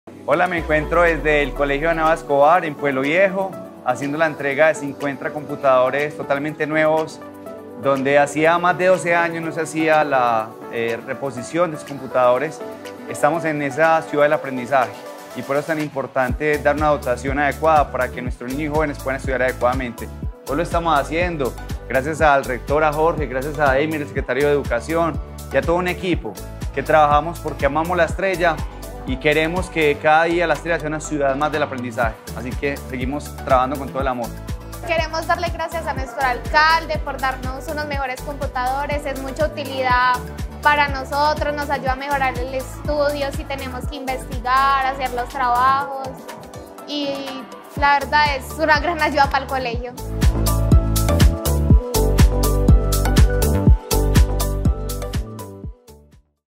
Nuevos computadores para la Ana Eva Escobar
Después de 12 años llegan nuevos computadores a la Ana Eva Escobar en Pueblo Viejo
👌🏼💥 En la construcción de una CiudadDelAprendizaje 💙🤍💚 estamos seguros que la tecnología 🌐 nos ayuda a crear espacios de aprendizaje más adecuados para los Siderenses.

Es por eso que la I.E Ana Eva Escobar ha recibido 🎉 50 computadores portátiles 💻 gracias a la gestión realizada con la 🤝Gobernación de Antioquia, que servirán para la creación de 2 salas de 👨🏻‍💻informática que beneficiará a un total de 3️⃣8️⃣0️⃣ estudiantes.

¡Somos una Secretaría imparable! ⚡⚡⚡